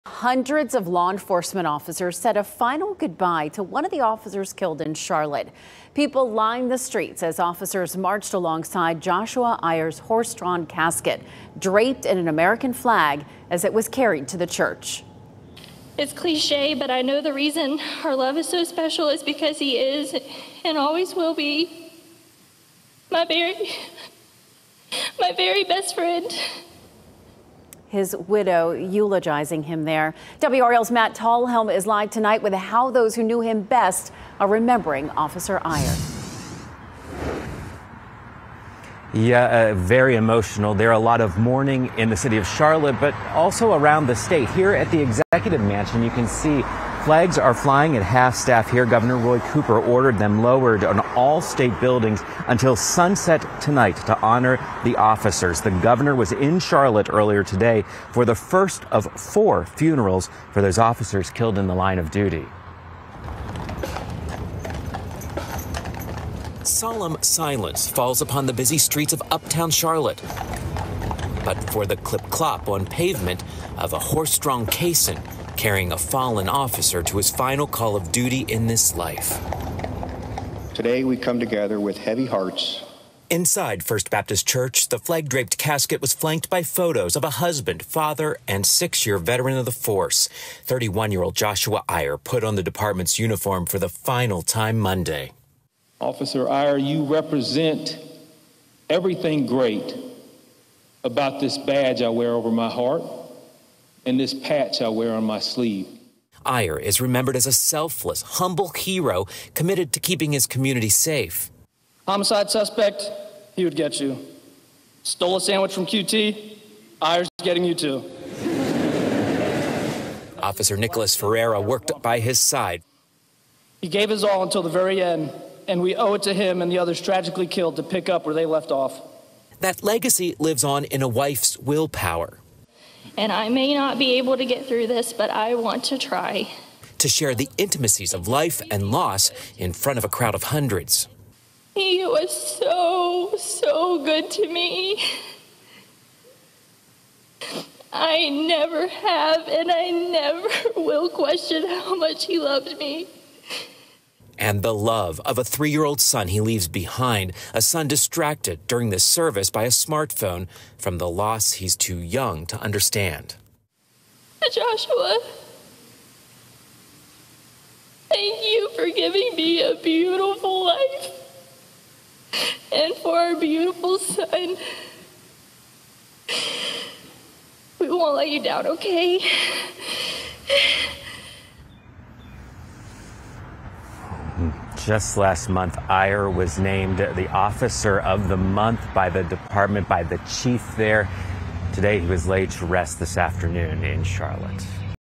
'A beautiful life:' Hundreds gather for memorial service for officer killed in Charlotte
Wife Ashley Eyer shared tearful memories of her husband, saying, "For 10 years, Josh and I built a beautiful life. I never have and never will question how much he loved me, and I will carry his love with me for the rest of my life."